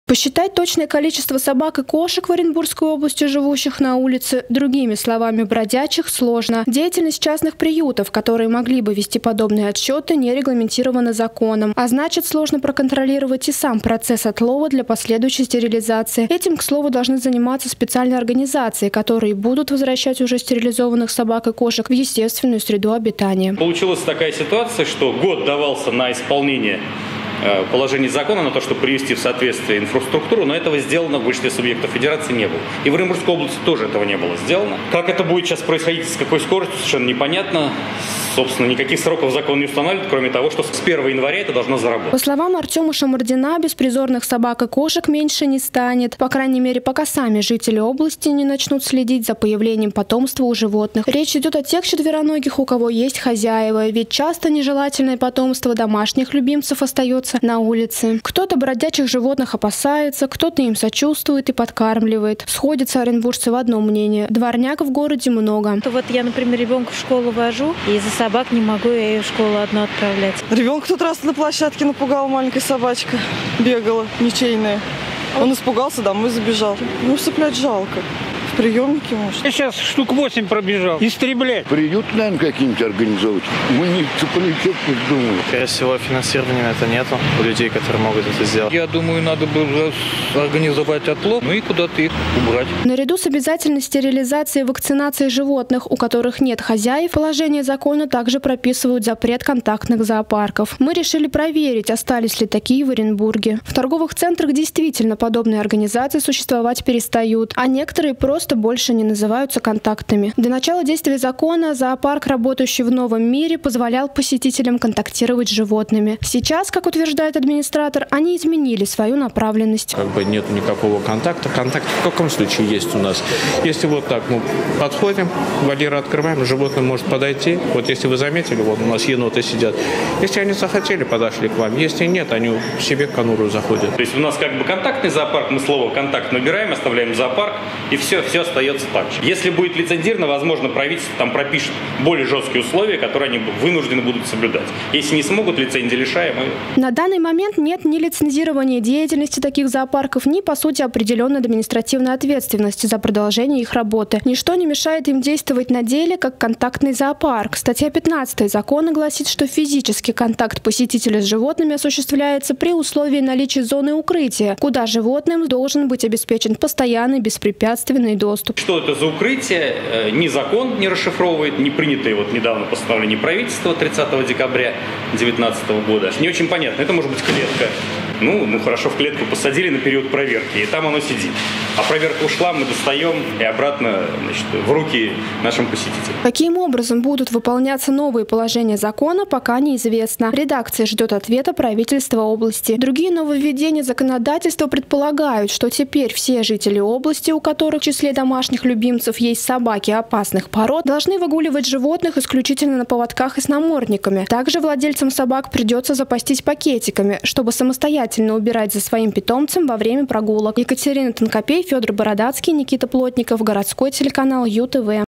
UTV. Как в Оренбургской области действуют нововведения Закона об ответственном обращении с животными
Структуру, которая бы занималась стерилизацией и вакцинацией беспризорных животных, в Оренбургской области так и не создали